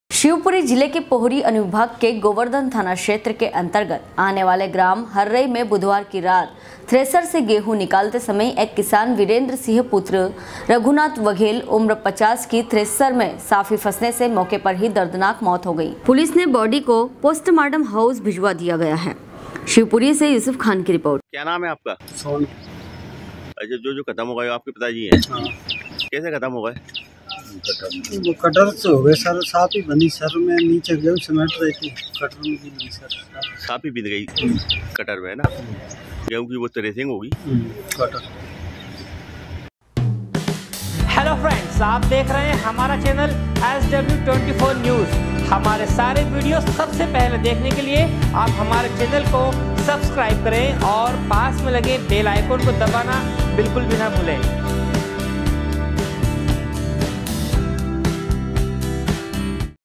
SW 24 News Madhyapradesh
शिवपुरी जिले के पोहरी अनुविभाग के गोवर्धन थाना क्षेत्र के अंतर्गत आने वाले ग्राम हर्रई में बुधवार की रात थ्रेसर से गेहूं निकालते समय एक किसान वीरेंद्र सिंह पुत्र रघुनाथ वघेल उम्र 50 की थ्रेसर में साफी फंसने से मौके पर ही दर्दनाक मौत हो गई। शिवपुरी से युसूफ खान की रिपोर्ट